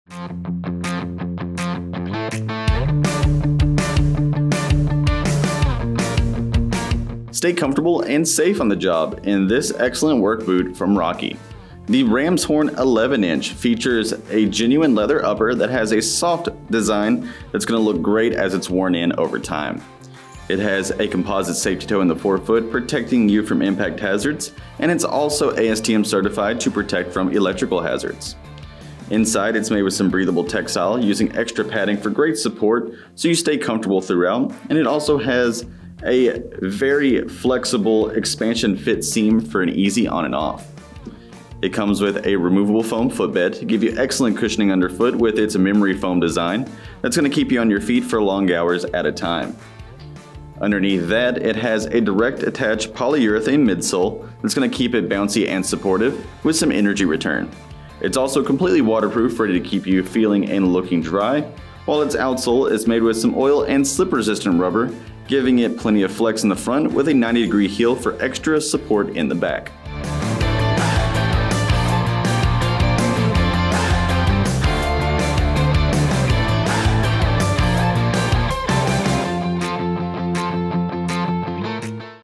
Rocky Rams Horn 11" Western Comp Toe SKU: 9861876
Designed with a versatile western-styled footwear and stitch detailing, the Rocky® Rams Horn 11" Western Comp Toe keeps him comfy.
Composite safety toe meets ASTM F2413-18 impact and compression, EH (Electrical Hazard) standard.
Abrasion resistant meets ASTM D1630-16 standard.
Melt point meets ASTM D276 standard.
Genuine leather upper.
Fabric lining.
Rocky® Rebound cushioning technology.
Flexible direct attached PU midsole.
Removable memory foam footbed.
Fiberglass shank for added support.
Expansion fit seam for easy on and off.
Style number: RKW0396 (Brown).
Oil-and-slip resistant rubber outsole meets ASTM F2913-19, ASTM D471-16A standard.
Imported.